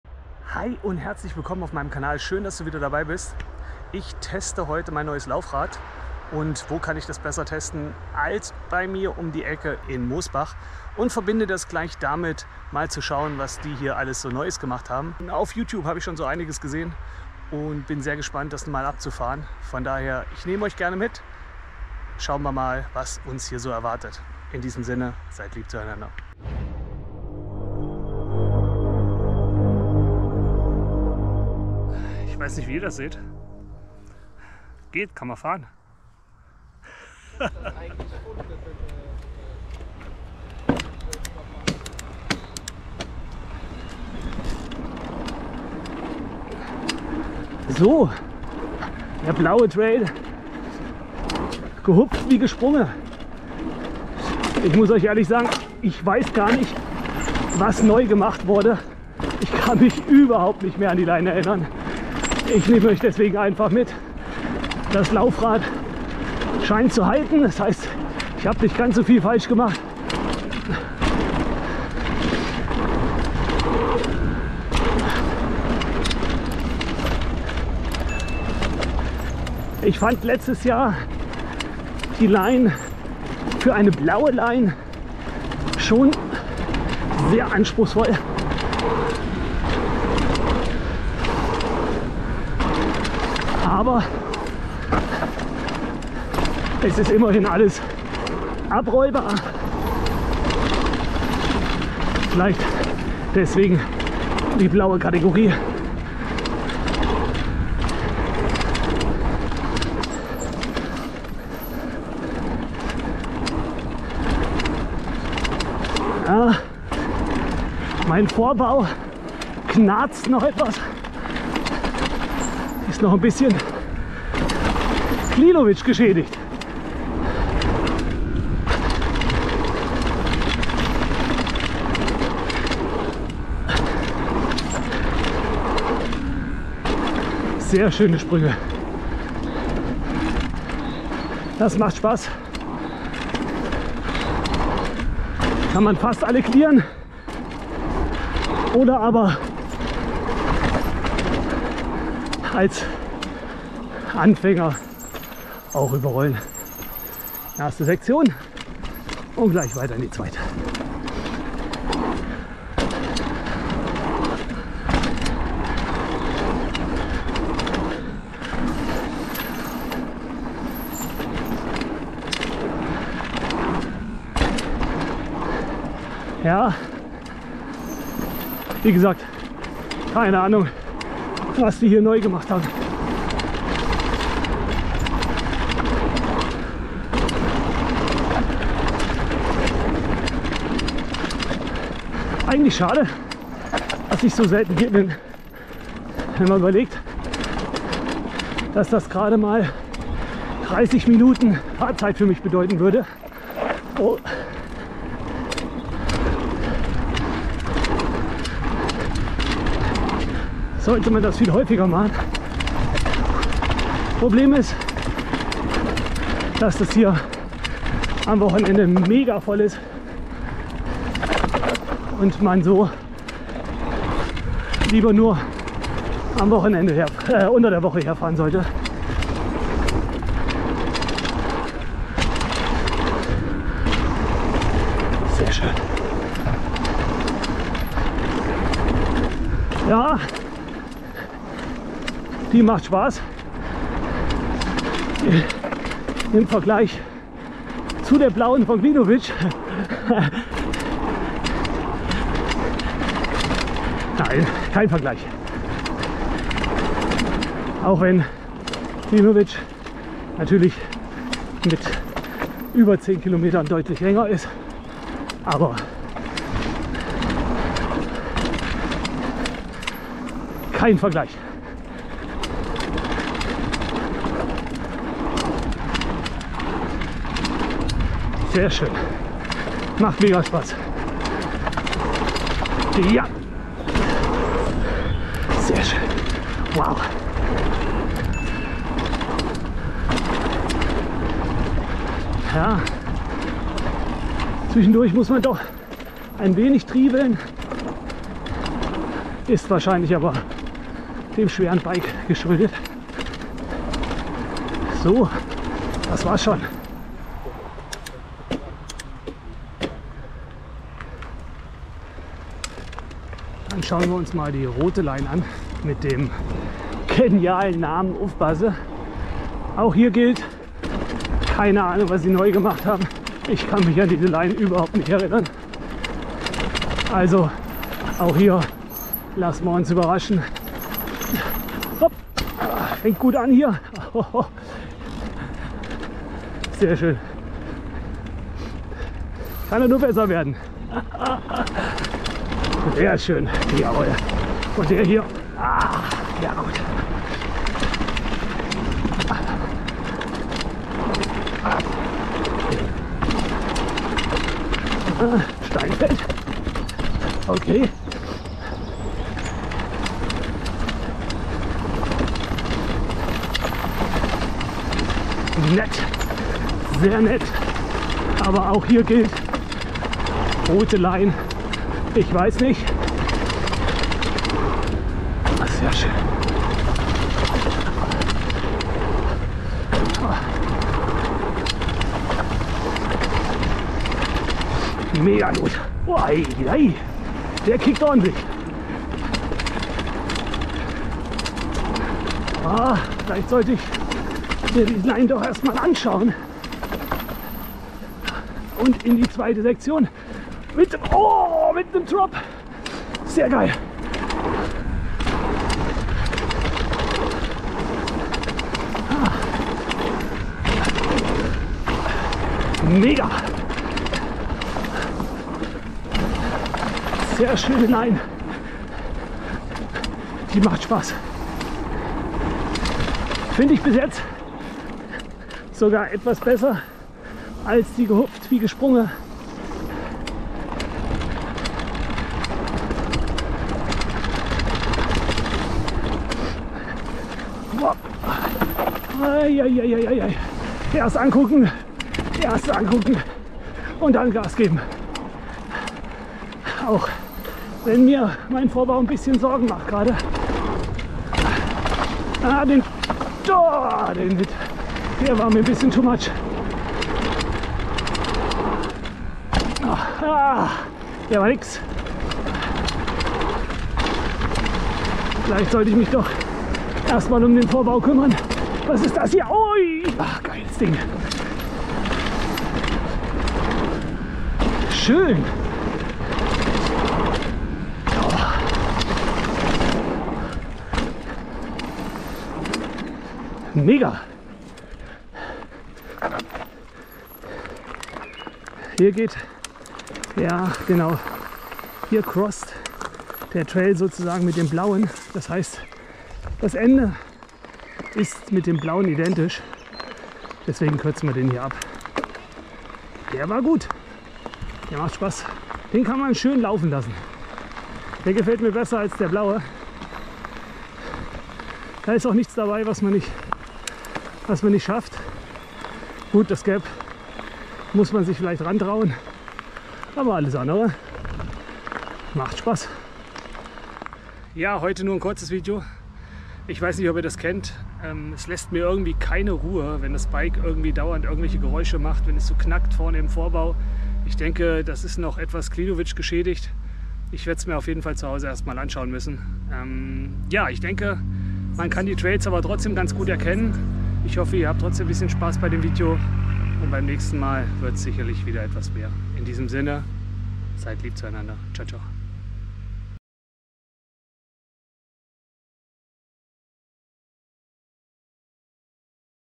EMTB - Flowtrail Mosbach - Laufradtest - Haibike AllMtn
Nachdem mein Laufrad in Klinovec eine kleine Auseinandersetzung mit einem etwas größeren Stein hatte, musste ein Neues her. Ich bin normalerweise was das Selberschrauben am Bike angeht mit zwei linken Händen gesegnet, so dass ich sehr gespannt war, ob das alles so Richtig war, was ich gemacht habe. Also nix wie nach Mosbach zum Testen.

Zwern:  @Zwern
Meike:  @meikestahl6006
Micha:  @emtbmicha
Olli:  @oliverdoerbeck9051
Carsten:  @m.i.c.a_adventure
Meme: @mcmemme
Maik:  @maikhirsmueller5865